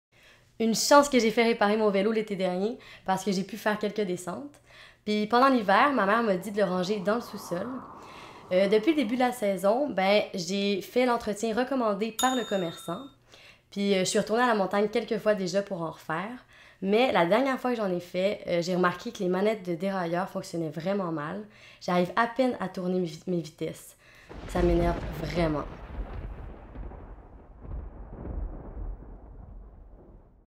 Capsule 5 / Les malheurs d'Alexie
Vidéo utilisée dans le cadre de la situation d'apprentissage les malheurs d'Alexie sur le

Scénario adapté d'une tâche de l'Office de la protection du consommateur.

Scénario :
Elle retourne voir le commerçant, qui lui apprend que la garantie du fabricant est terminée. « J’ai acheté une garantie supplémentaire », lui rappelle Alexie, qui demande ainsi au commerçant de réparer le vélo sans frais. Le commerçant lui apprend toutefois que la pièce n’est pas couverte par la garantie. Selon lui, le problème n’est pas lié à une défectuosité, mais à un mauvais usage de la part d’Alexie. Celle-ci sait pourtant qu’elle a pris grand soin de son vélo et qu’elle l’a peu utilisé. De plus, un spécialiste du vélo lui a confirmé qu’il s’agissait bien d’une défectuosité du produit. Stupéfaite, Alexie ne sait plus quoi faire. Le commerçant lui demande de payer 75 $ pour la réparation. Alexie a beau tenter de faire prévaloir son point de vue auprès du commerçant et de négocier avec lui, rien n’y fait. Elle n’a plus confiance en ce commerçant, ni en son vélo.